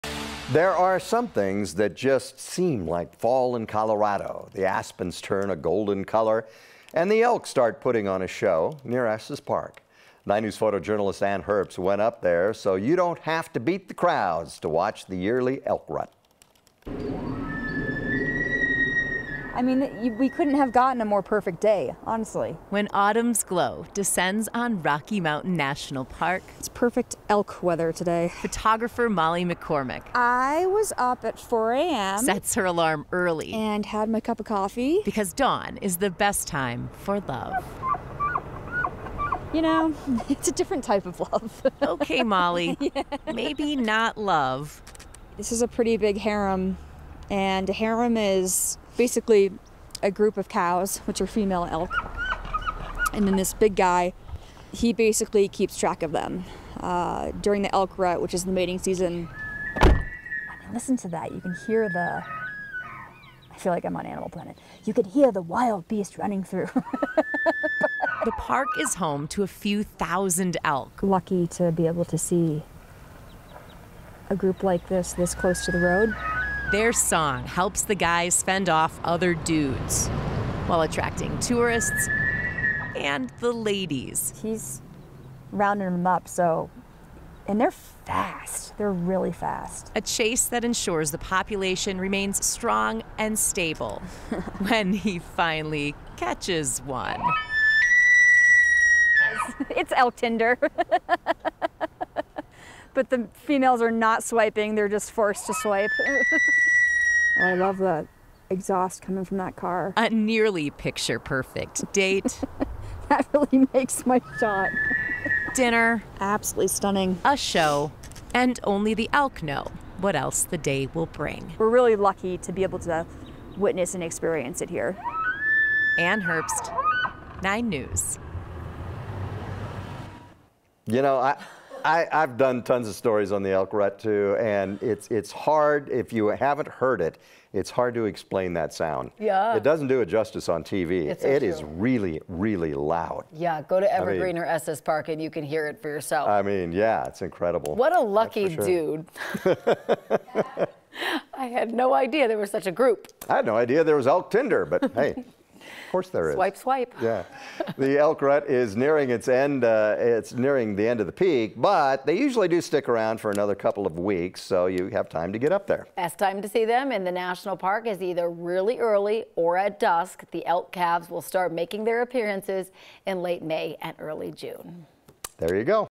It's elk rut season in Rocky Mountain National Park
When autumn's glow descends on Rocky Mountain National Park, photographer Molly McCormick sets her alarm early to take in the sights of elk rut season.

9NEWS (KUSA) is located in Denver, Colorado.